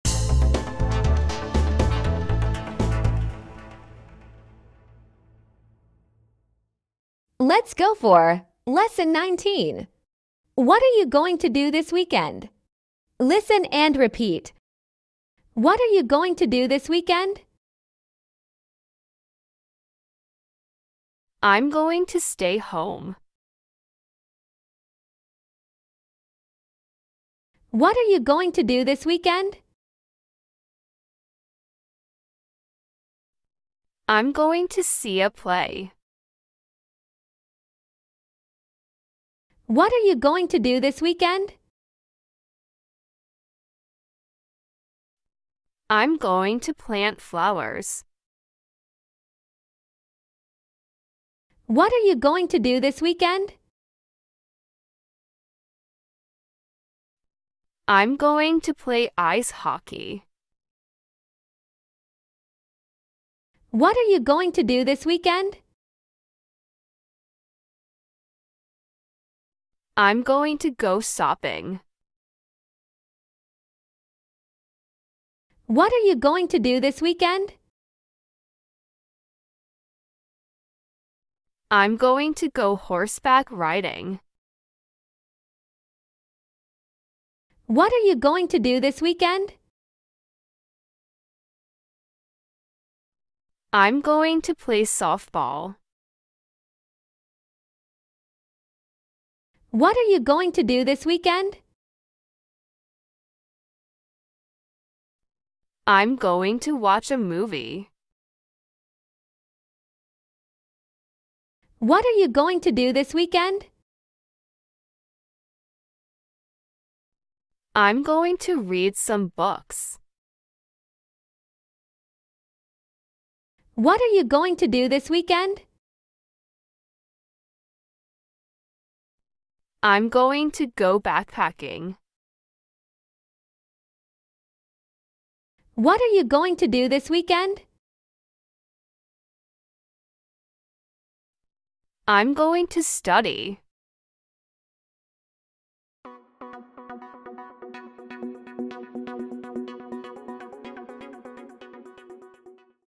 Let's Go 4 LESSON 19 | Talking About Future Plans | Weekend Activities for Kids! ESL DIALOGUE
🌟 Welcome to the Let's Go Series! In this exciting episode, we practice the dialogue "What are you going to do this weekend? I'm going to..." and explore common weekend activities. This video is perfect for young learners looking to improve their English by learning how to talk about future plans using 'going to'.

🎉 Join us as we dive into fun and engaging dialogues, discussing weekend plans like going to the park, visiting grandparents, playing sports, and more! This video is a fantastic way to enhance vocabulary and practice future tense in a fun and interactive way.

Hit play now and start planning your weekend in English!